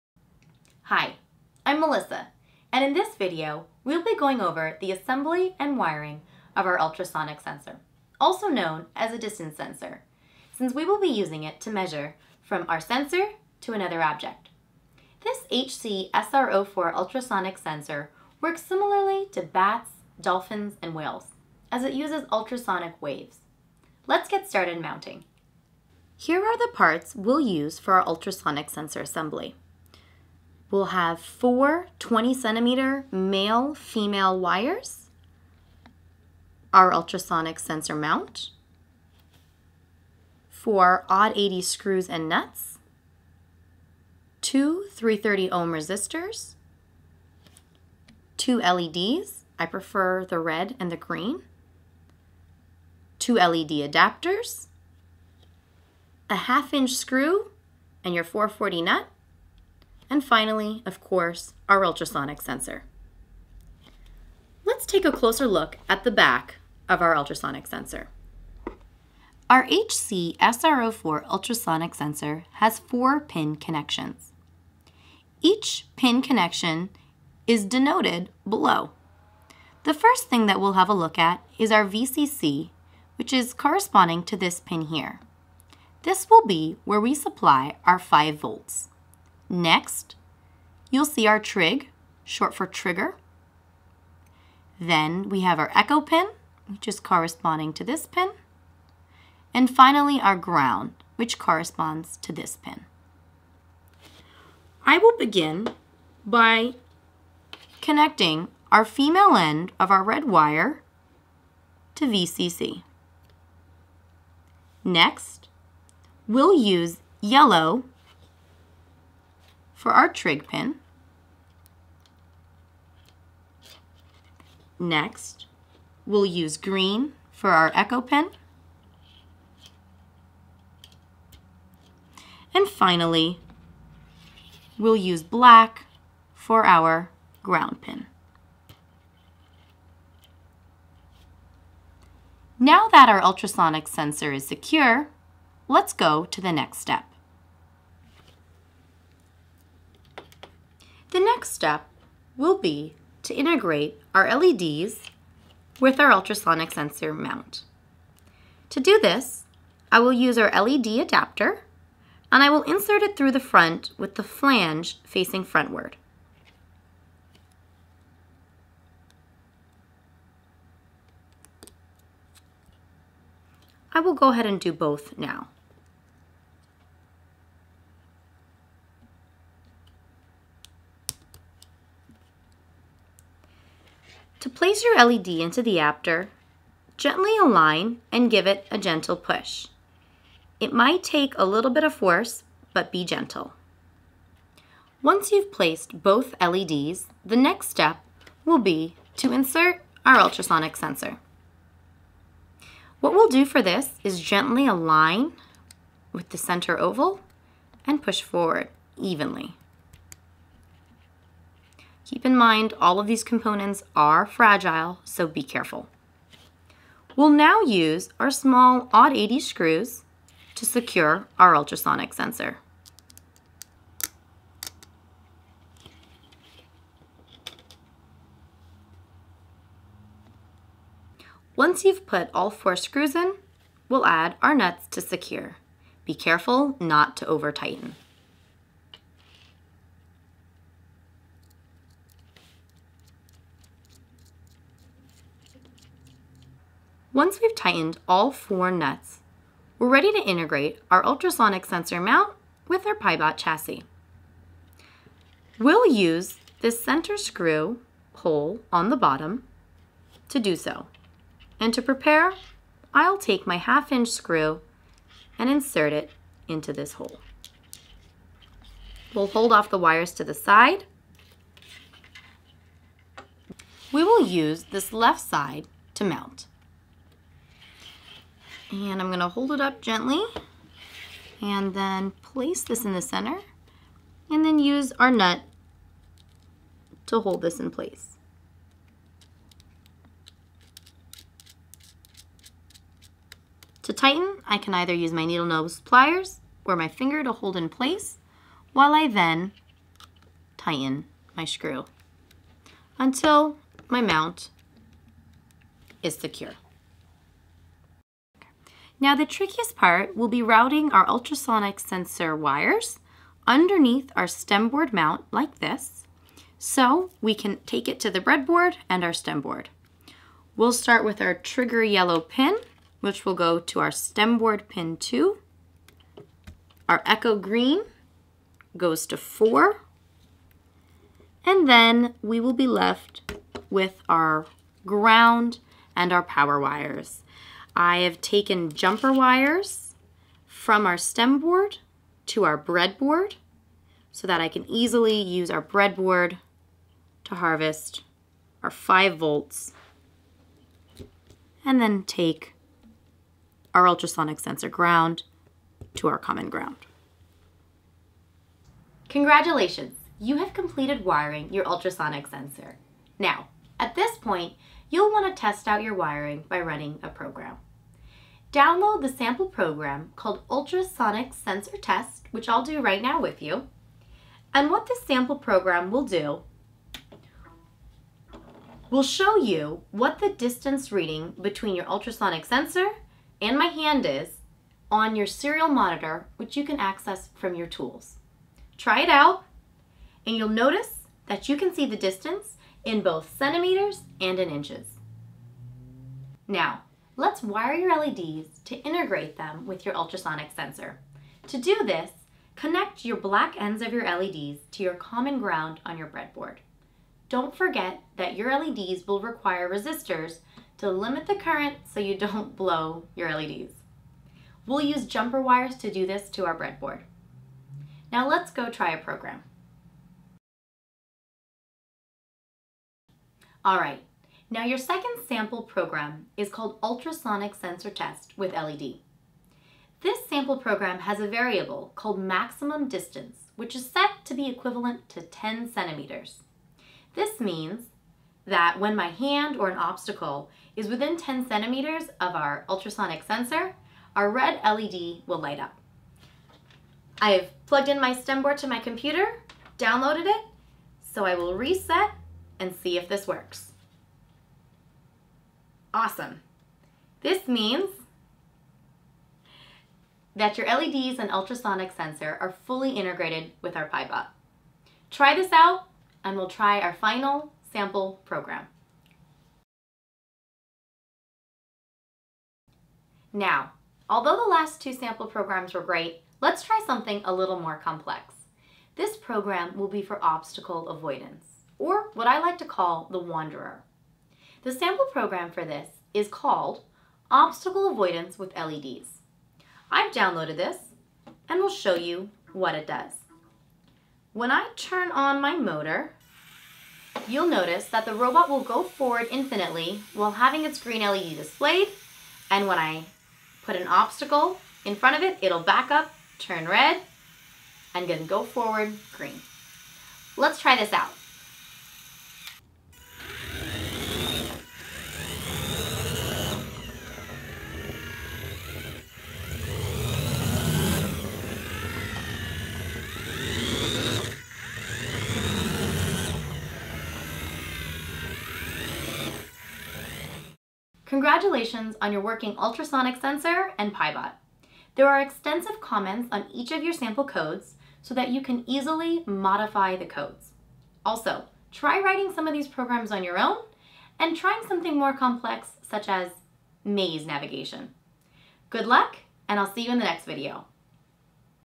Pi-Bot 8: Assembly, Wiring and Programming HC-SR04 Ultrasonic Sensor
In this short video, Melissa Jawaharlal shows how to assemble, wire and program the HC-SR04 ultrasonic sensor with your Pi-Bot. 3 sample programs are discussed as well - have fun and good luck!